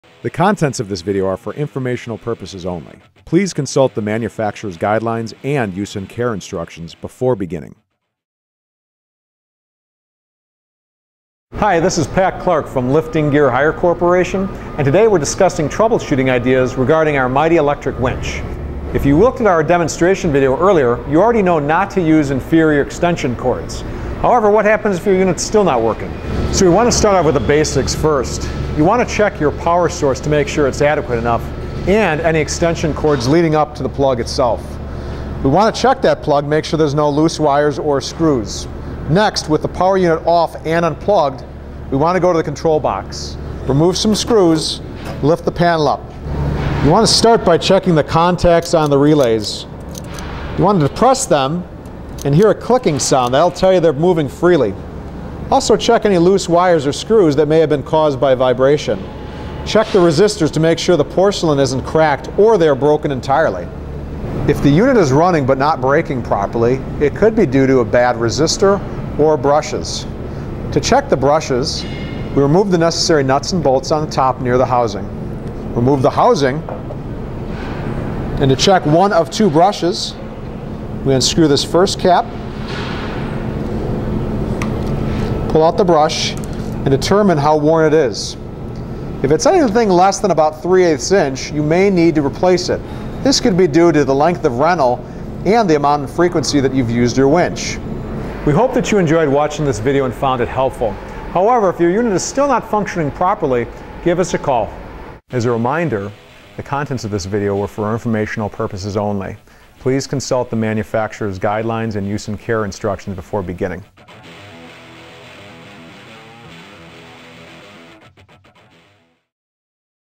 Troubleshooting Electric Winches
When electric winches stop working, it can cause a huge headache. In this troubleshooting video, LGH’s very own Pat Clark offers up a few quick, easy solutions to solve an electric winch failure.

Your next lifting solution!

To learn more about air-operated winches or get a quote for one, visit RentLGH.com